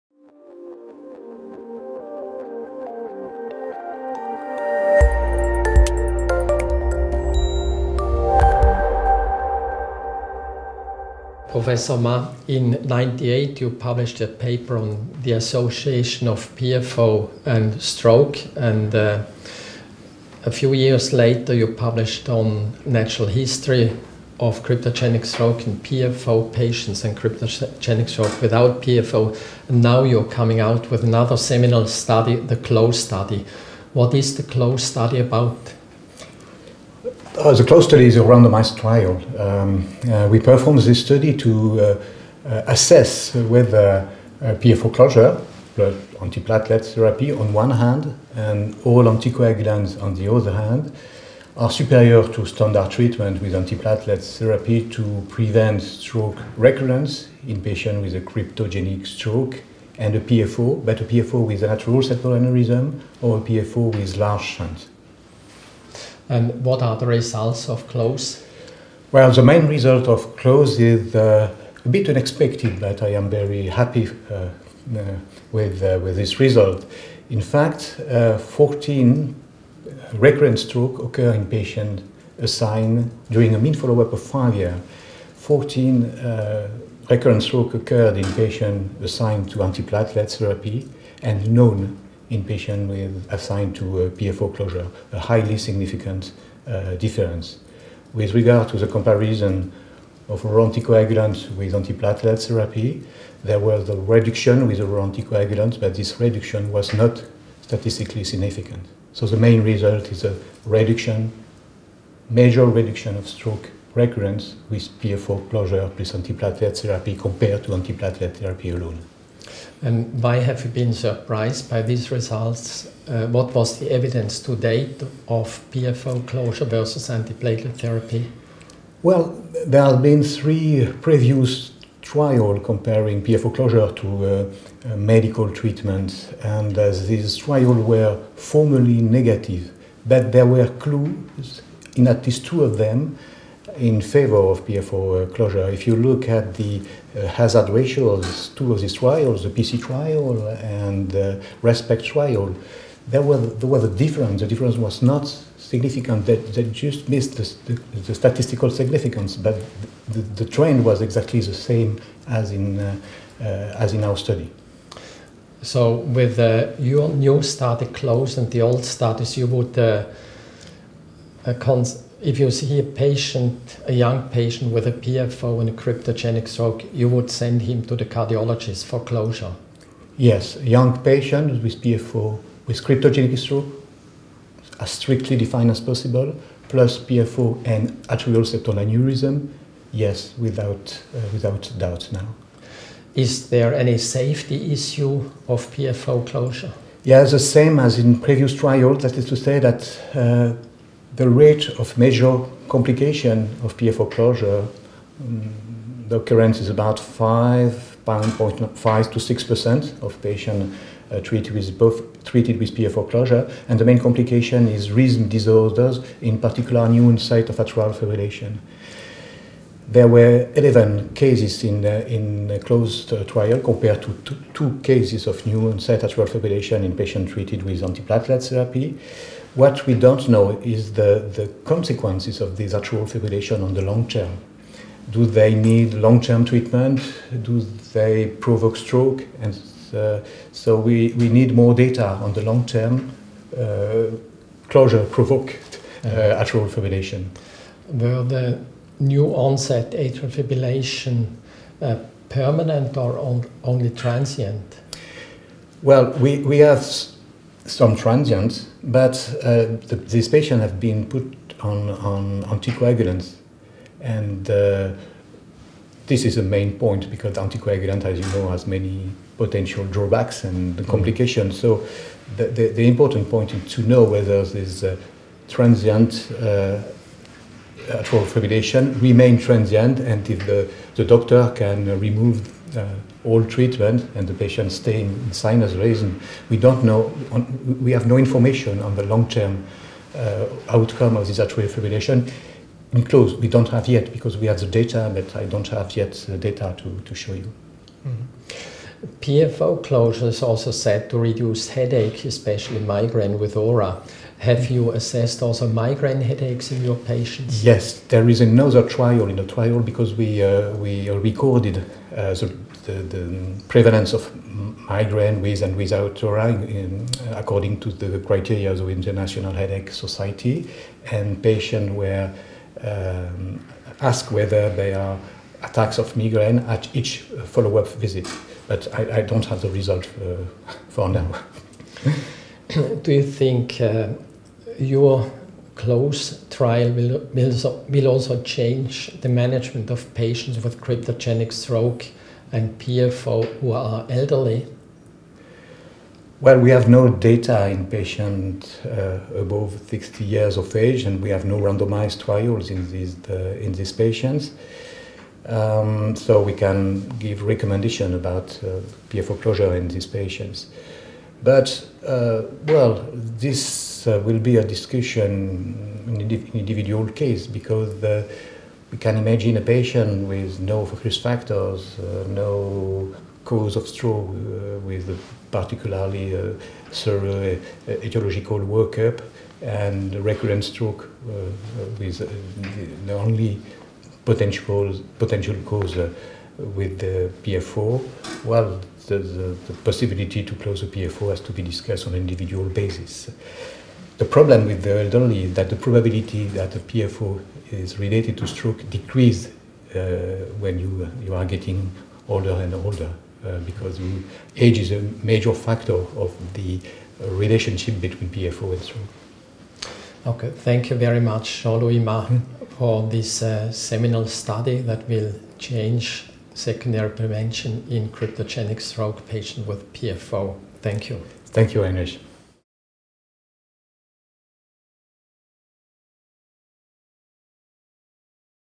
Jean-Louis Mas talks about the CLOSE Trial at ESOC 2017
Jean-Louis Mas talks to Heinrich Mattle about the CLOSE Trial at ESOC 2017 in Prague.  To assess whether (1) PFO closure with device plus (chronic) antiplatelet therapy on one
hand, and (2) oral anticoagulants on the other hand, are superior to antiplatelet therapy, to
prevent stroke recurrence in patients 16 to 60 years old with cryptogenic stroke and PFO
with atrial septal aneurysm or PFO with large shunt.